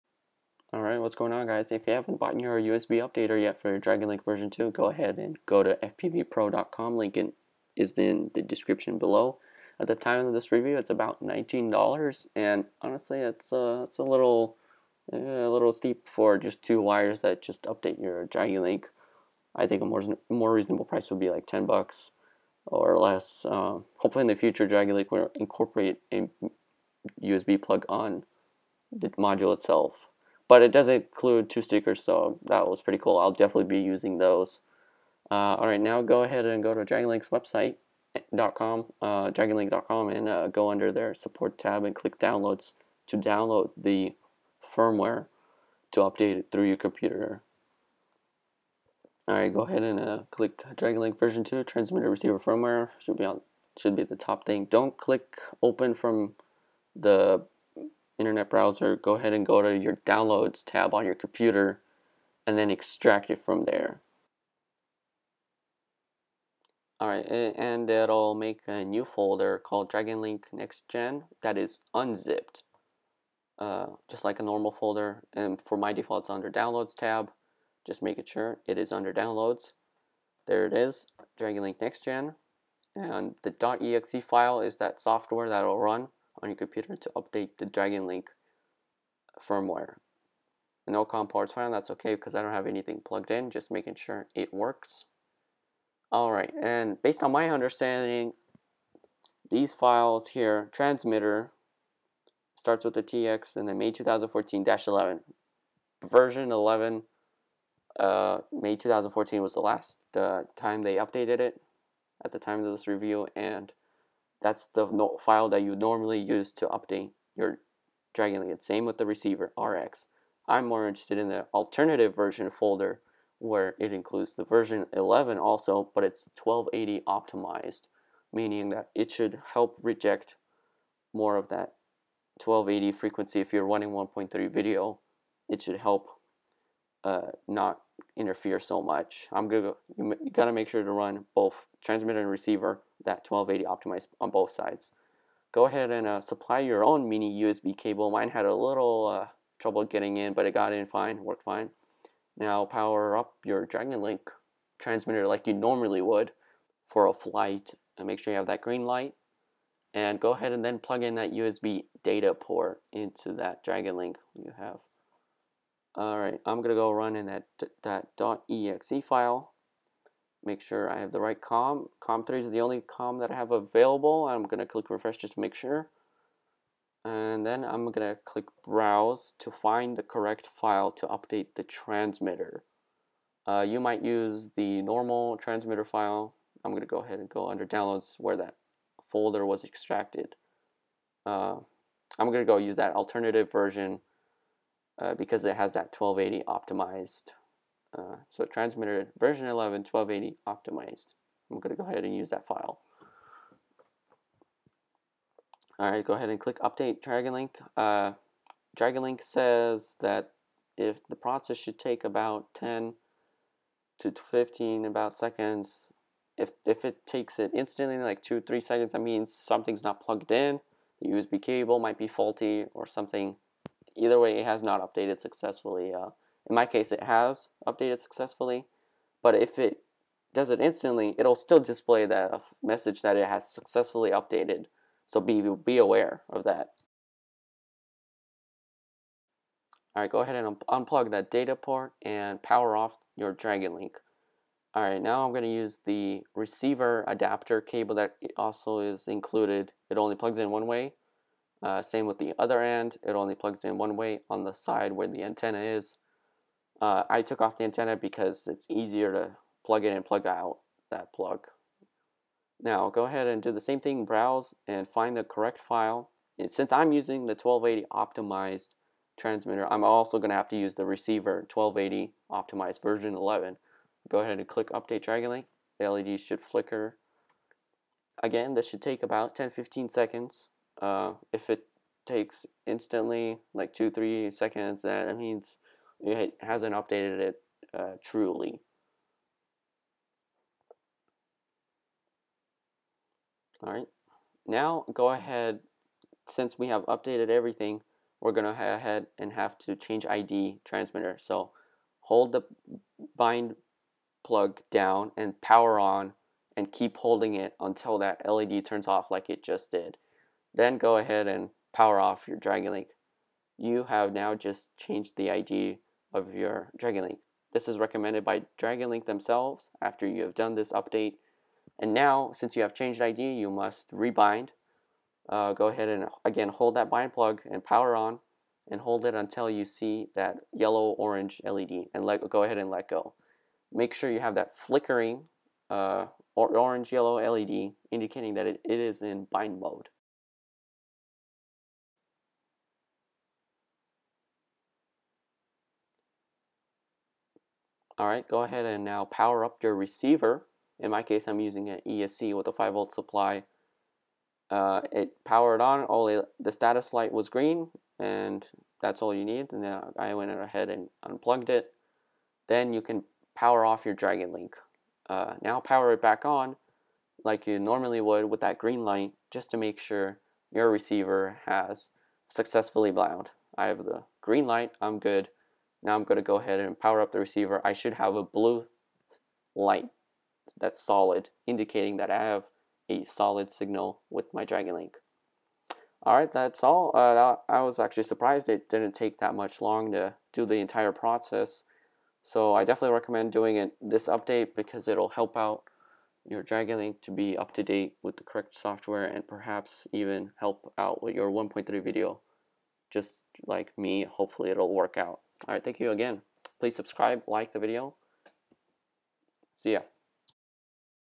Update Your DragonLink V2
A step by step process of how to update your DragonLink V2 with the USB updater using the firmware provided by DragonLink.com. A big recommendation from me to update your DragonLink for the new benefits and better stability.